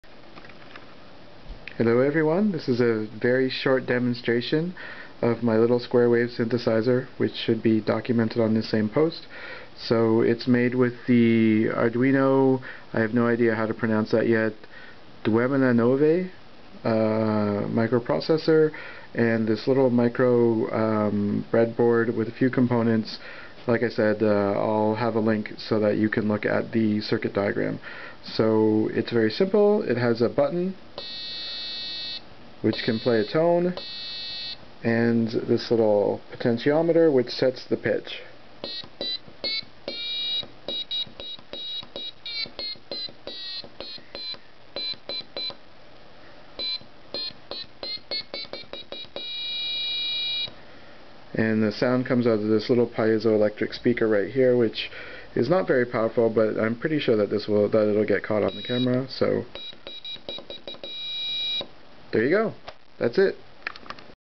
First Arduino Project: Simple Square Wave Synth
This is my first attempt at putting together an Arduino project.  I've had the thing about 20 minutes.  :)  (Well, by now a few hours, since it took me a while to document the project, but you get the idea.)  This was made by putting together the ideas from a few of the Arduino example projects.  This video is just a quick demo.  For project documentation, feel free to go here: